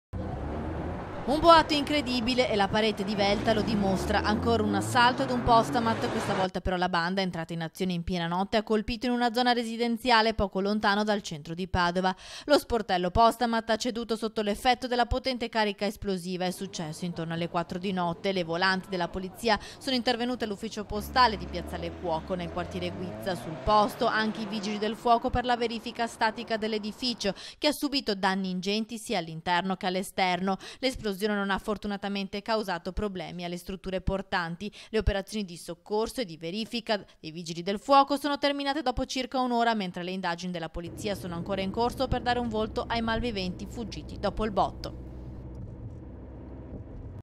TG PADOVA (04/05/2019) - ESPLODE BANCOMAT IN PIENA ZONA RESIDENZIALE
TG PADOVA (sabato 4 maggio 2019) - Un boato incredibile. E il parete divelta lo dimostra. Ancora un assalto ad un postamat questa volta però la banda entrata in azione in piena notte ha colpito in una zona residenziale poco lontano dal centro di Padova. Lo sportello Postamat ha ceduto sotto l'effetto della potente carica esplosiva. È successo intorno alle 4 della notte: le volanti della polizia sono intervenute all'ufficio postale di piazzale Cuoco nel quartiere Guizza. Sul posto sono giunti anche i vigili del fuoco per la verifica statica dell'edificio, che ha subito ingenti danni interni ed esterni: l'esplosione non ha fortunatamente causato problemi alle strutture portanti. Le operazioni di soccorso e verifica dei vigili del fuoco sono terminate dopo circa un’ora, mentre le indagini della polizia sono ancora in corso per dare un volto ai malviventi, fuggiti dopo il botto.